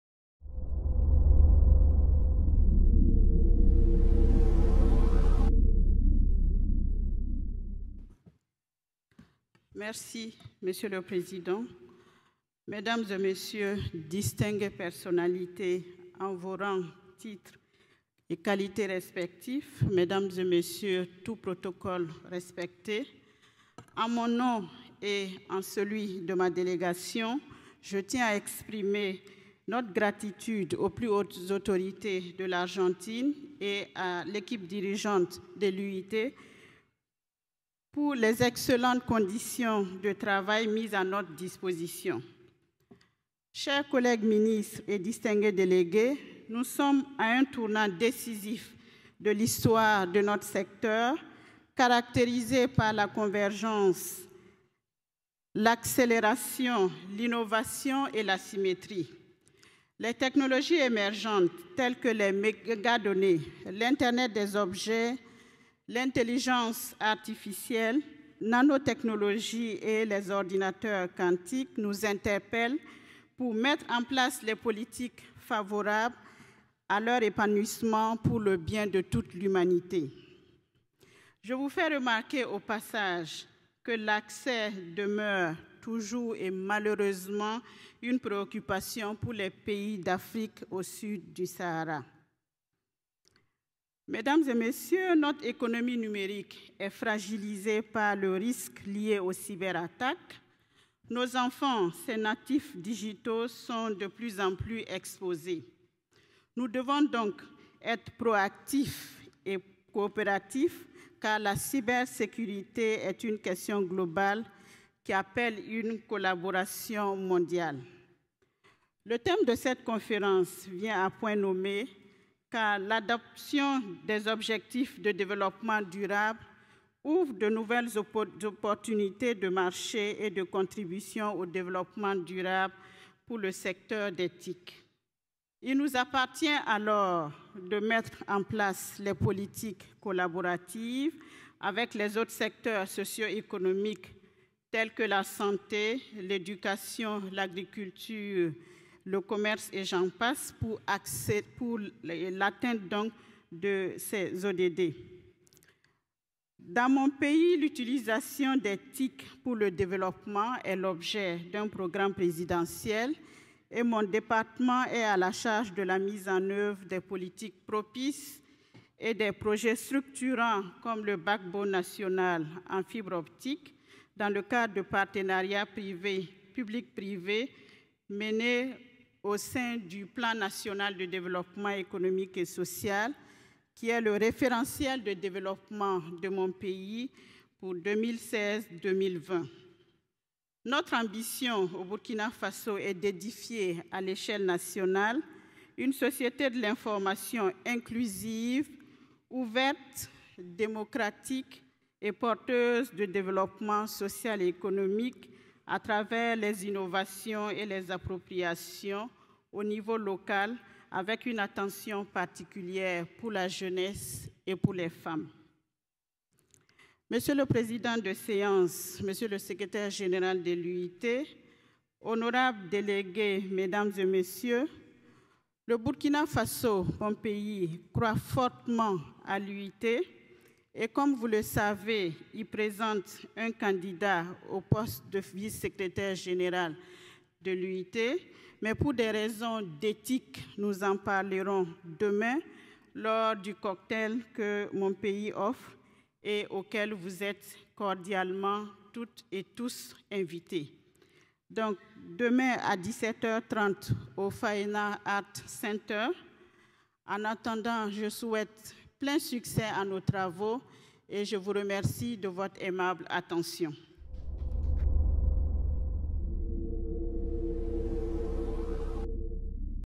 WTDC-17: H.E. Ms Hdja Fatimata Ouattara/Sanon Ministre du Développement de l'économie numérique
Policy Statement at WTDC-17 by H.E. Ms Hdja Fatimata Ouattara/Sanon, Ministre du Développement de l'économie numérique et des Postes, Burkina Faso